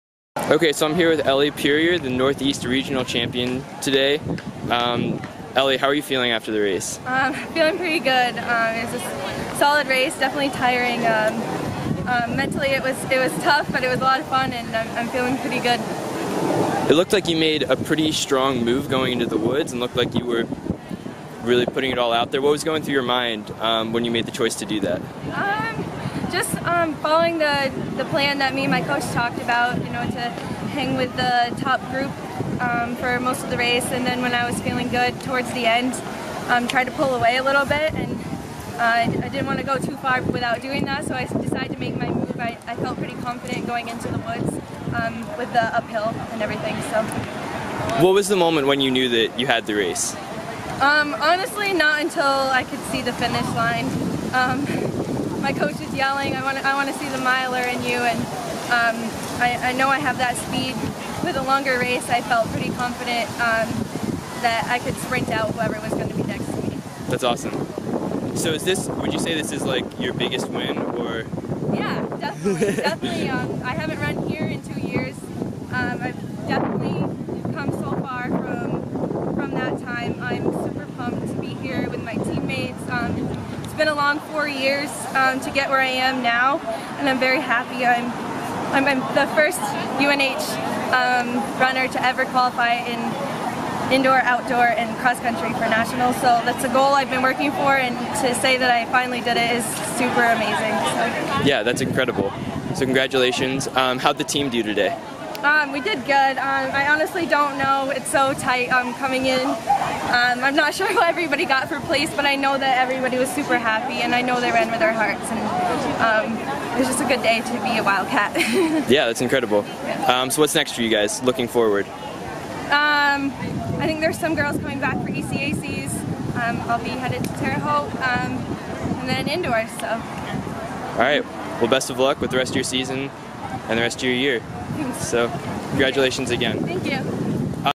Elinor Purrier after winning NCAA Northeast region
New Hampshire's Elinor Purrier after winning NCAA Northeast region. D1Northeast results